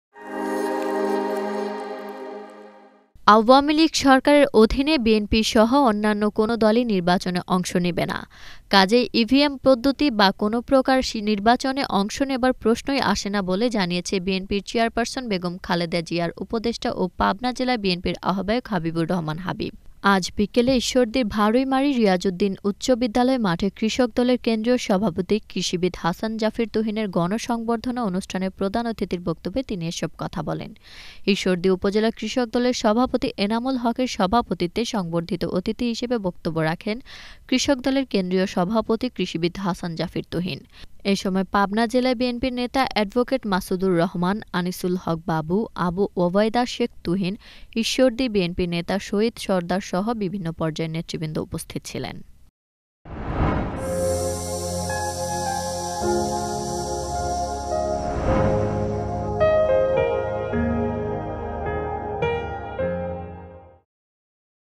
আওয়ামীলীগ সরকারের অধিনে বিএনপিসহ কোন দলই নির্বাচনে অংশ নিবেনা| Channel S News | Channel S 2022
আওয়ামীলীগ সরকারের অধিনে বিএনপিসহ অন্যান্য কোন দলই নির্বাচনে অংশ নিবেনা। কাজেই ইভিএম পদ্ধতি বা কোন প্রকার নির্বাচনে অংশ নেবার প্রশ্নই আসেনাবলে জানিয়েছেন, বিএনপির চেয়ারপার্সন .

1: Ajker Desh Jure News :
2: Channel S Rater Songbad News :